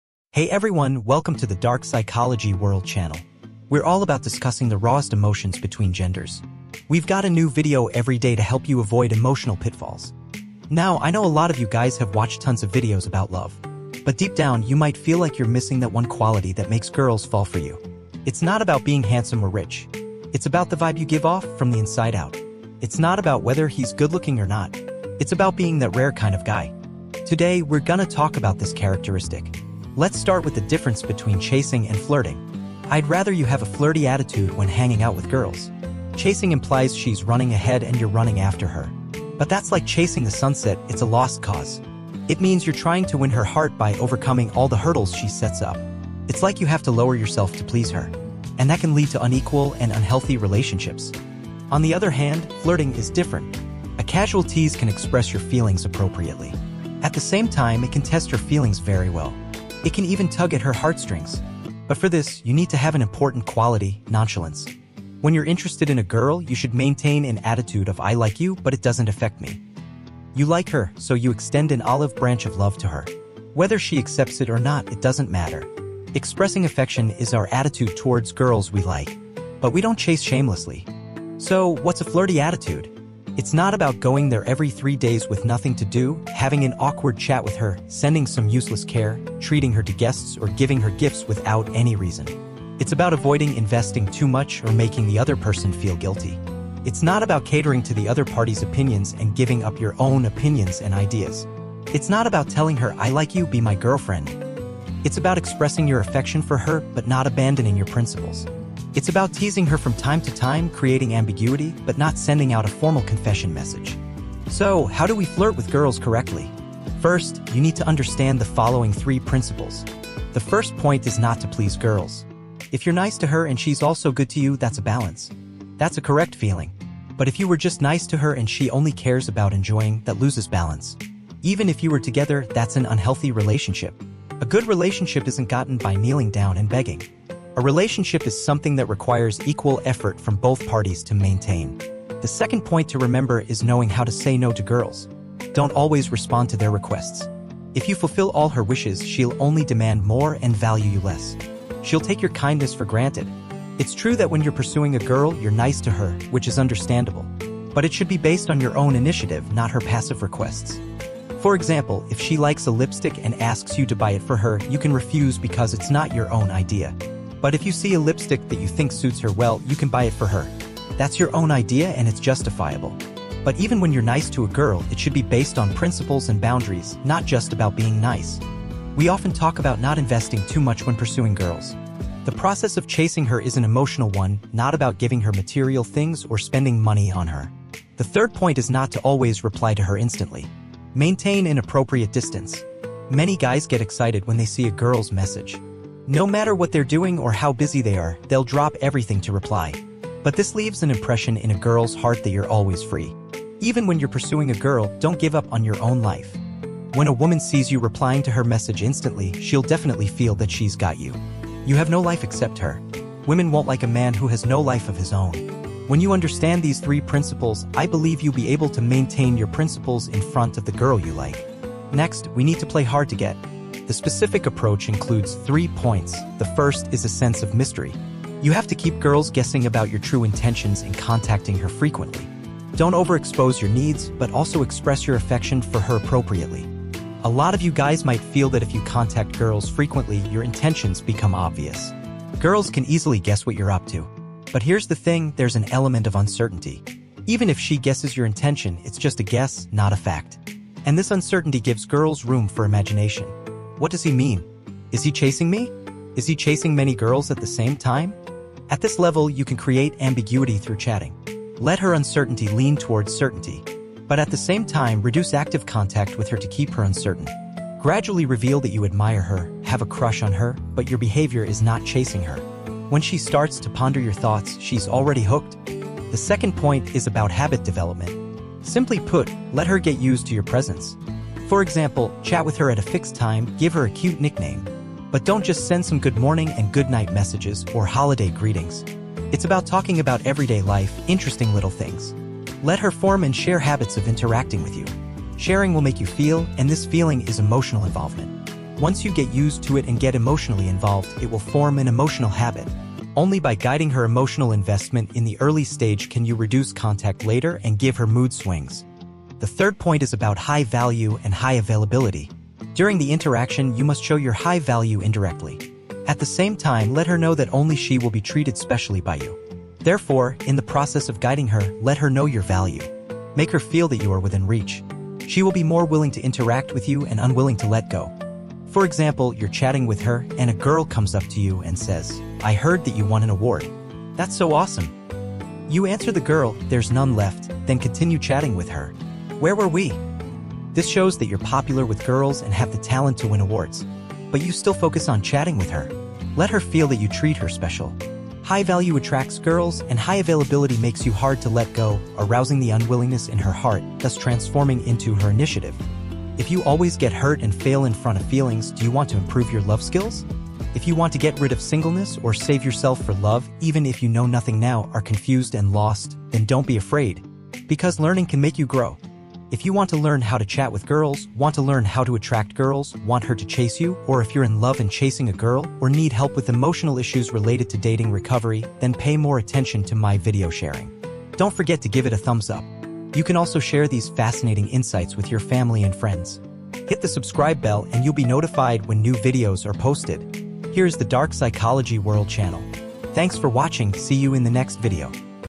Unlock the Mystery of Attraction: Flirting vs. Chasing
Welcome to another episode on Dark Psychology World! Today, we delve into the rawest emotions between genders and the art of attraction. We’re not talking about being the most handsome or the richest. We’re talking about that rare quality that makes girls fall for you - the vibe you give off from the inside out. Join us as we explore the difference between chasing and flirting, and how to maintain a flirty attitude without losing your principles. Remember, a good relationship requires equal effort from both parties. So, sit back, relax, and let’s avoid those emotional pitfalls together!